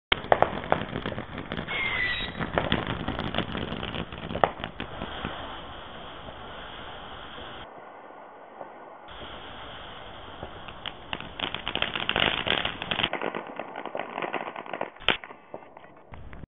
Fireworks 2009 Video 5
Some Footage I shot of my Fireworks going off.  Video had to Rotated 90 Degrees and resized.  Two Reasons for this: the fireworks would have looked like they were going off Horizontally, and to have a 4:3 aspect ratio instead of a 3:4 aspect ratio.